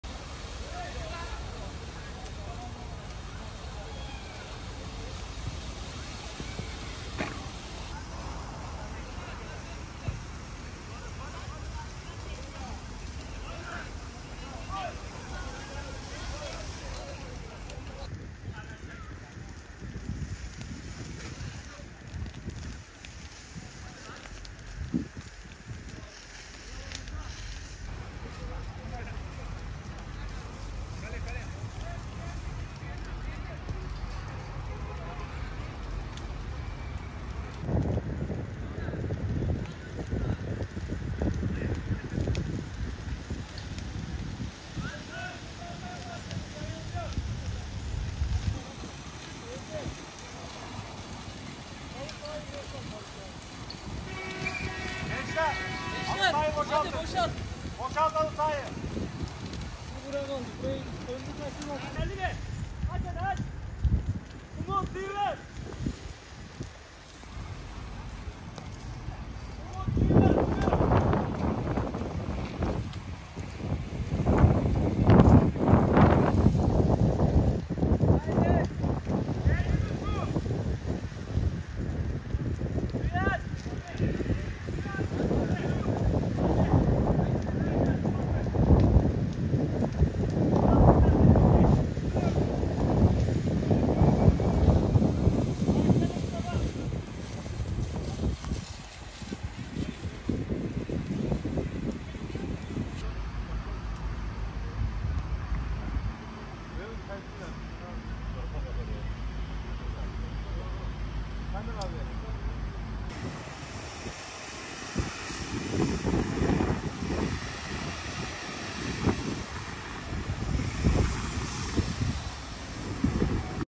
Çerkezköy’de korkutan yangın: Dere kenarında çıkan yangın ağaçlara sıçradı
Çerkezköy’de dere kenarındaki otluk alanda başlayıp ağaçlara sıçrayan yangın itfaiye ekiplerince söndürüldü.